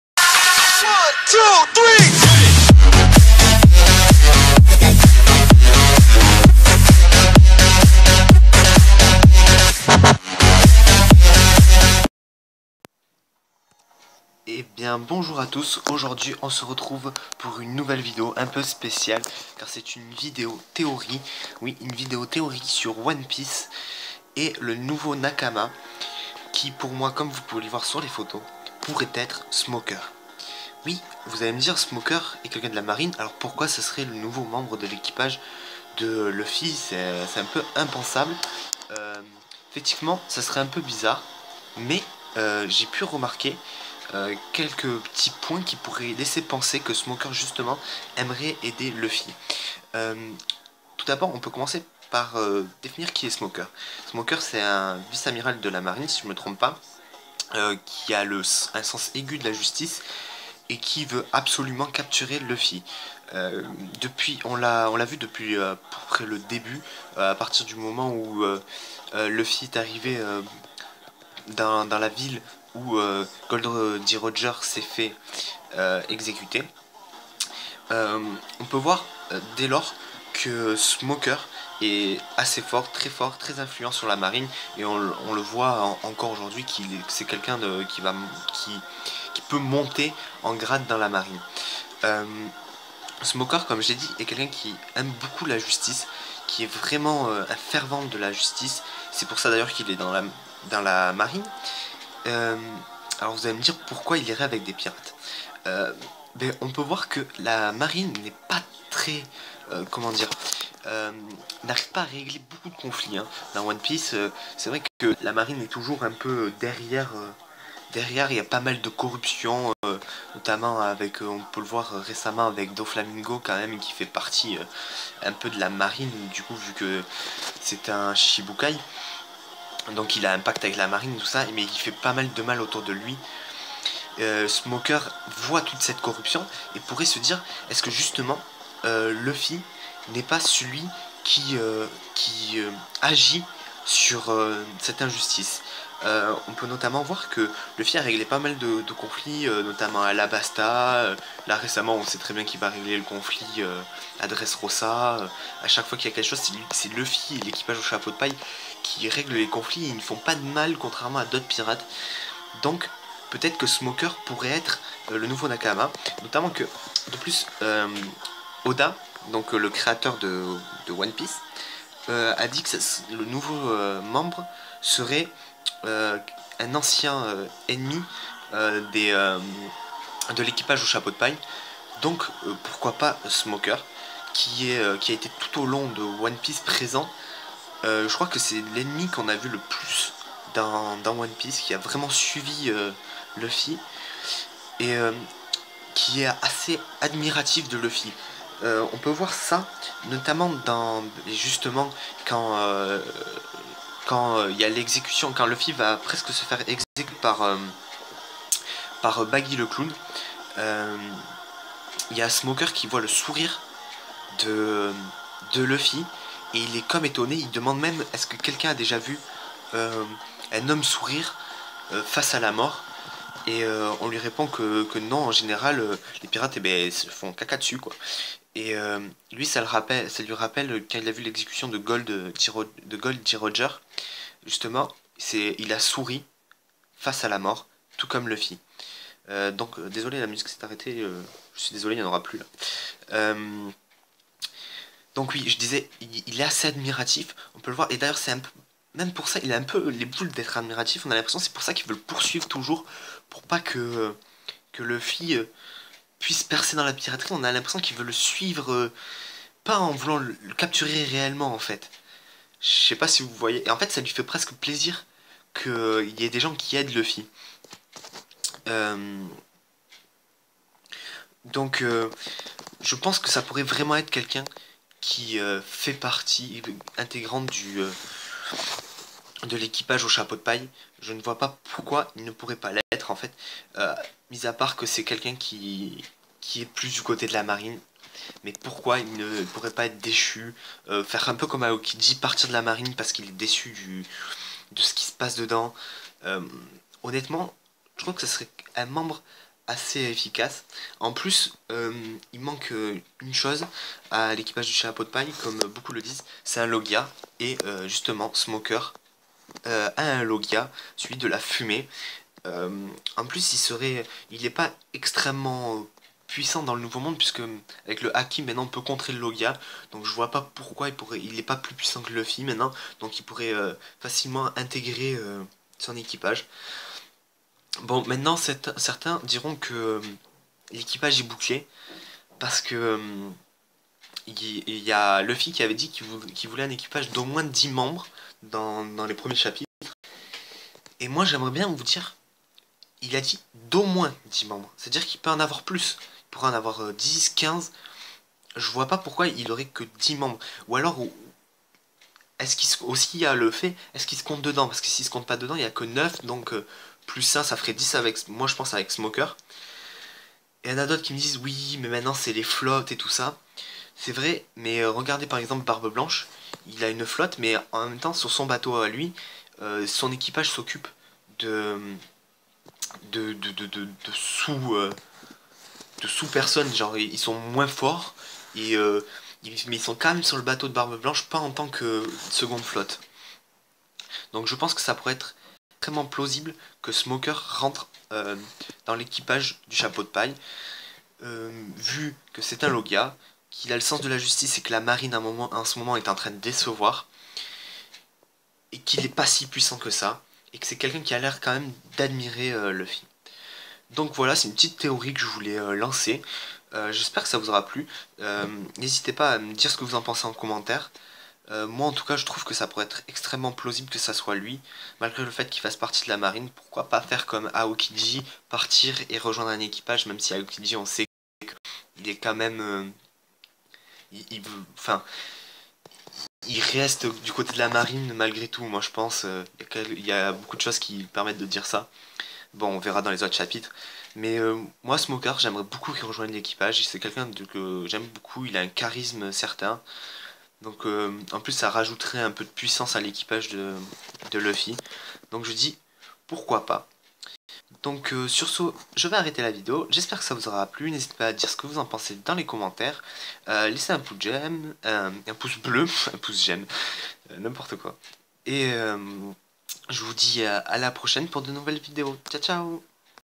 One piece theory # Smoker le prochain nakama ?
Une vidéo théorie sur un manga populaire One piece.